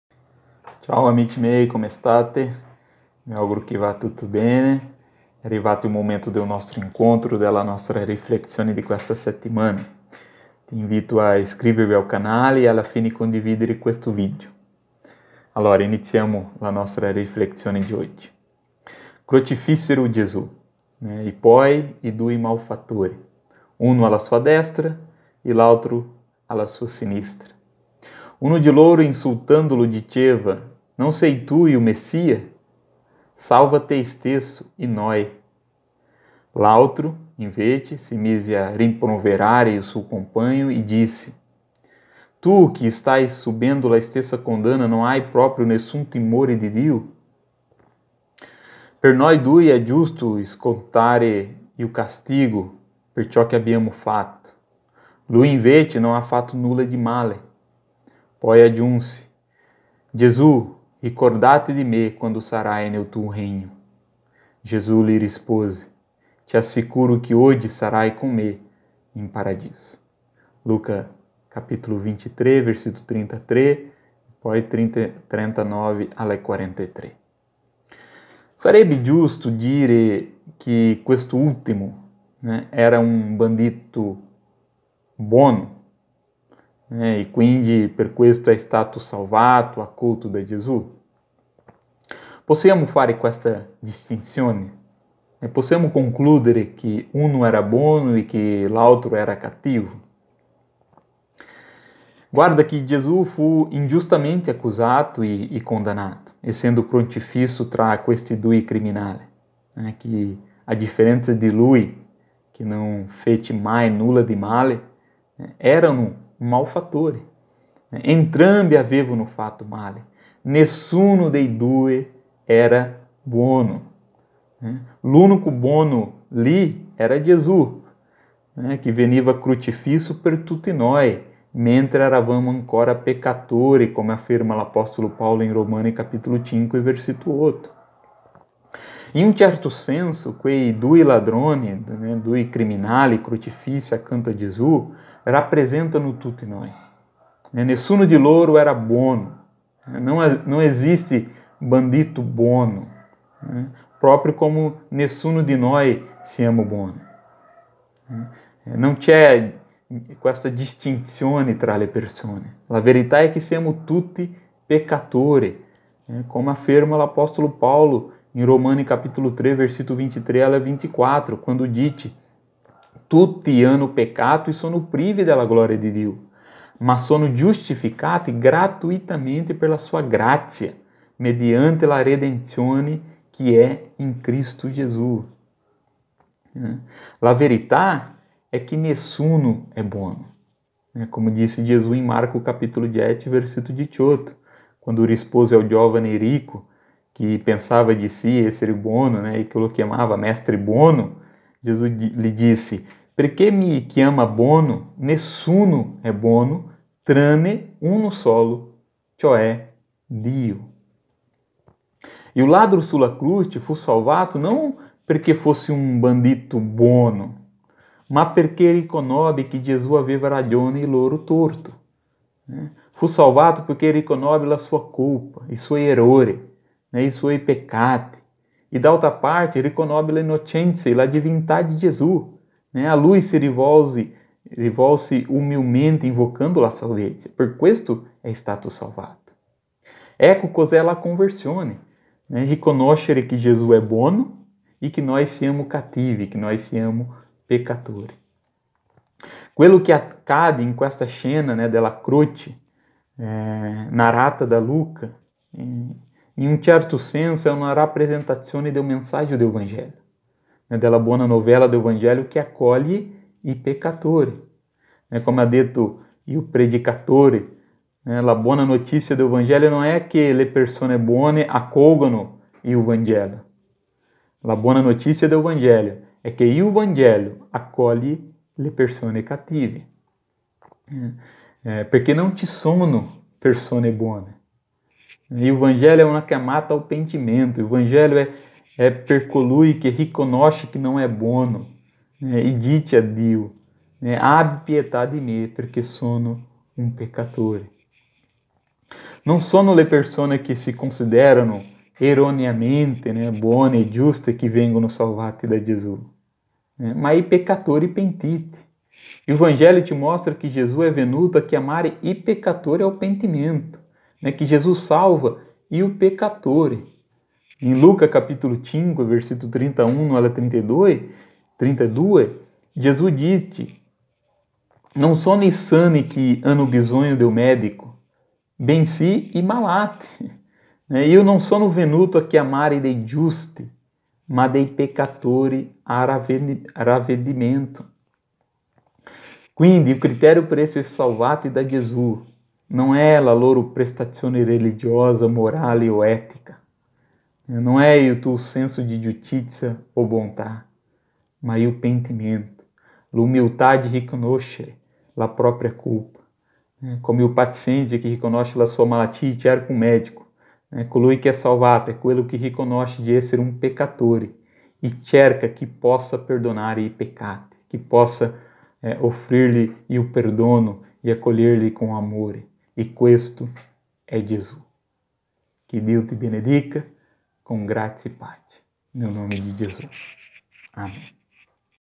Il bandito buono!?
“Per noi due è giusto scontare il castigo per ciò che abbiamo fatto, lui invece non ha fatto nulla di male. Poi aggiunse: - Gesù, ricordati di me quando sarai nel tuo regno. Gesù gli rispose: - Ti assicuro che oggi sarai con me in paradiso” (Luca 23.41-43)